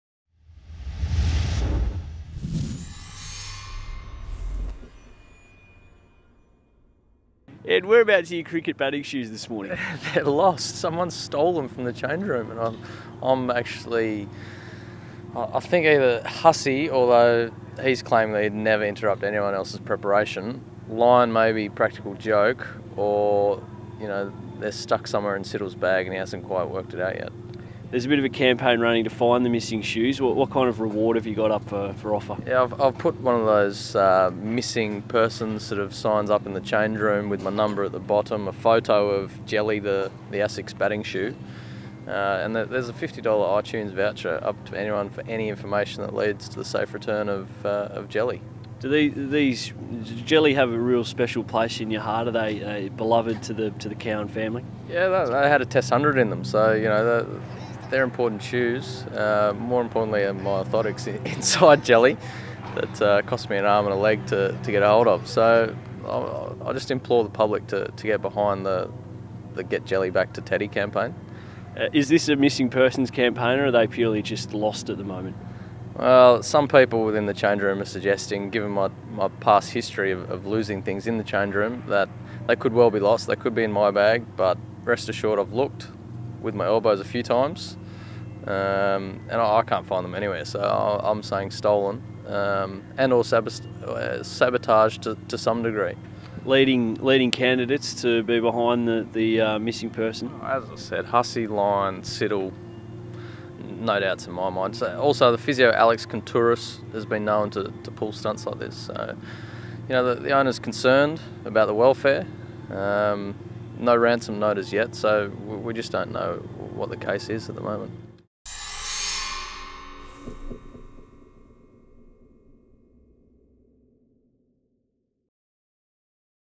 Have you seen Ed's shoes?
Ed Cowan chats to CATV about his missing shoes and his campaign to find them again!